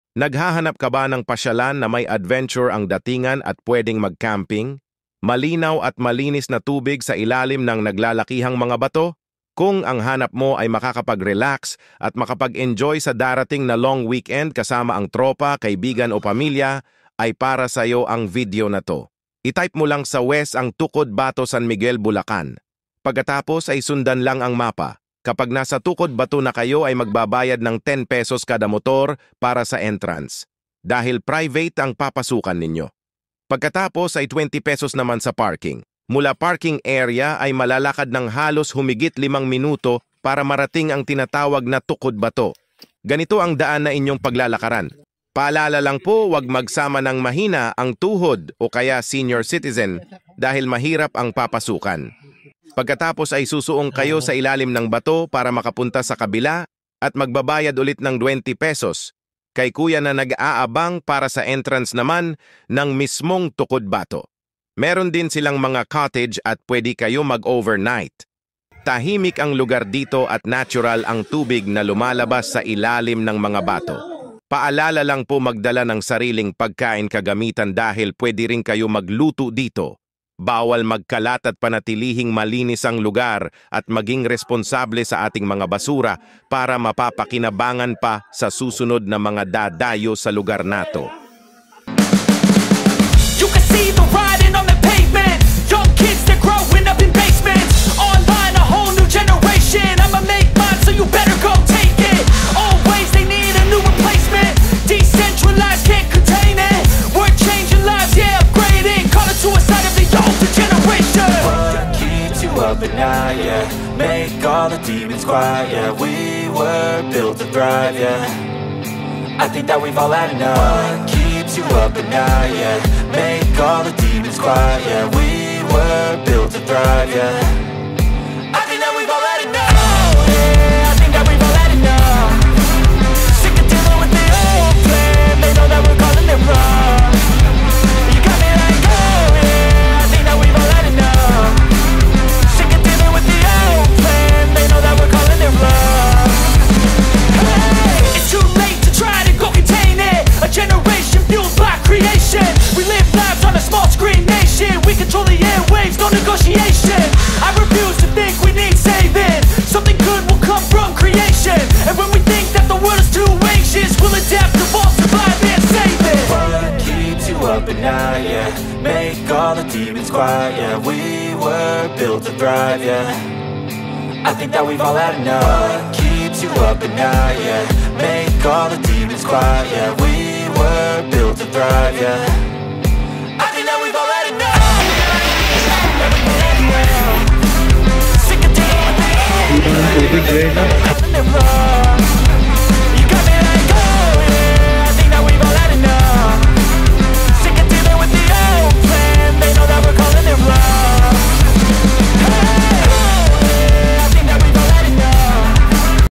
Pinakamagandang nature spring | TUKOD NA BATO San Miguel Bulacan
Tag init na kaya kailagan mo na ng relaxing na nature spring. Tubig na nangggaling sa ilalim ng naglalakihang mga bato. Malinis at malinaw kaya sobrang marerelax ka dito